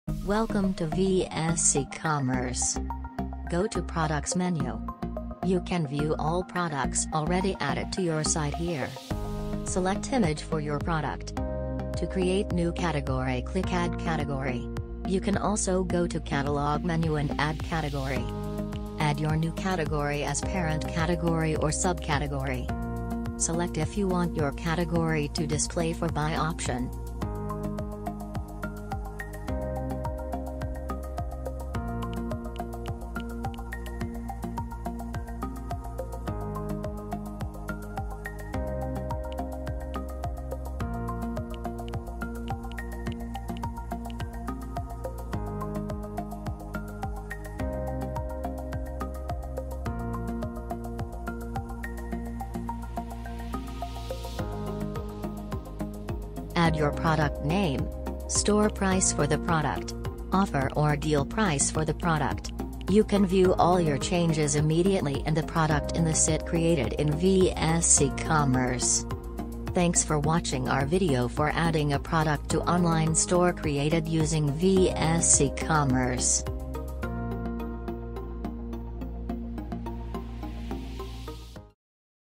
vsEcom - How to add a product in vsecommerce
This video explains, how to add a product in vsEcommerce. Using our VS ecommerce you can create online store instantly
Welcome to VS ecommerce
Go to products menu
You can view all products already added to your site here
Select Image for your Product
To create new category click Add category
You can also got to Catalog menu and add category
Add your new category as Parent Category or Sub Category
Select if you want your category not to display for Buy option

Add your Product Name
Store Price for the product
Offer or Deal price for the Product
You can view all your changes immediately and the product in the sit created in VS ecommerce